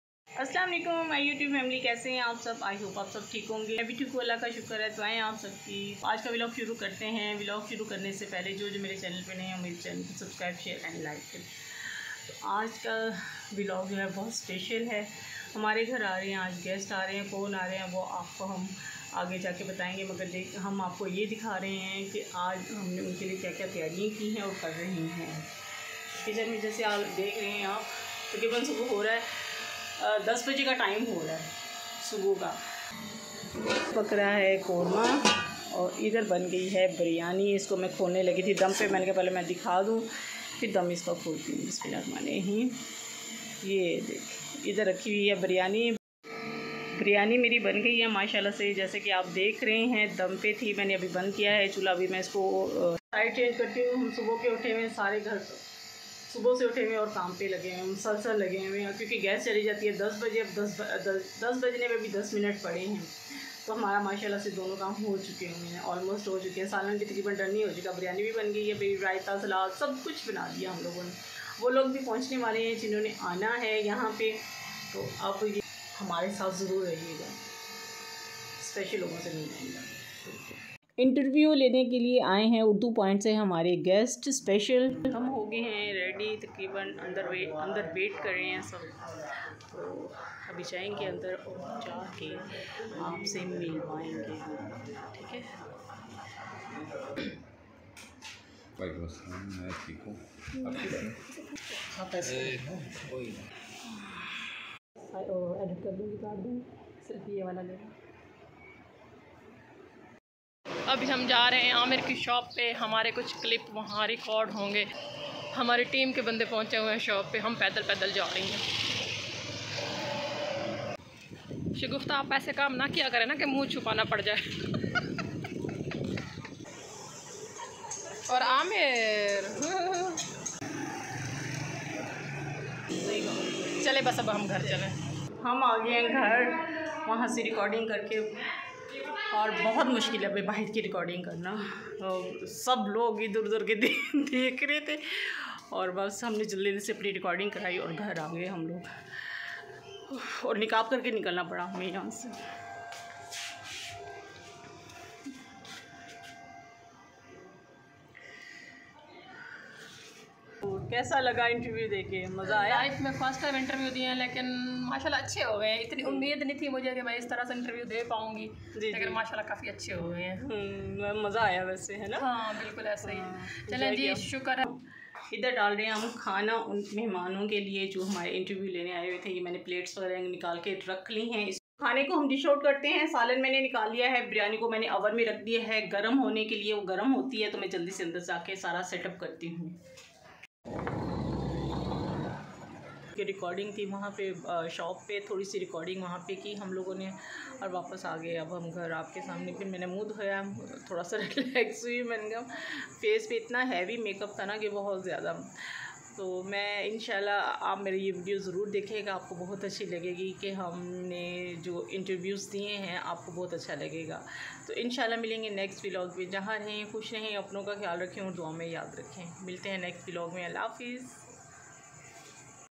Urdu Point Pe Interview 😊 | Shagufta jabeen | Routine vlog
Urdu Point Pe Interview 😊
Urdu Point Anchor Muhammad Akmal has interviewed them.What the love story on this trio' house wife Youtuber | hard work Mom | lunch time | tasty biryani | with korma | and much more fun |
maine first time interview Diya Hai
bahut achcha Laga interview de kar
Urdu point Multan
Shagufta jabeen 2nd wife  Mahnoor Aamir and my husband name Muhammad Aamir
enjoy interview.
interesting story 😂.
Urdu Point ko see off Kiya
interview dene shop par bhi gey
outdoor recording |
My daily routine | busy mom | active mum |  thank you for Urdu point  | very enjoy  the interview

Thanks 😊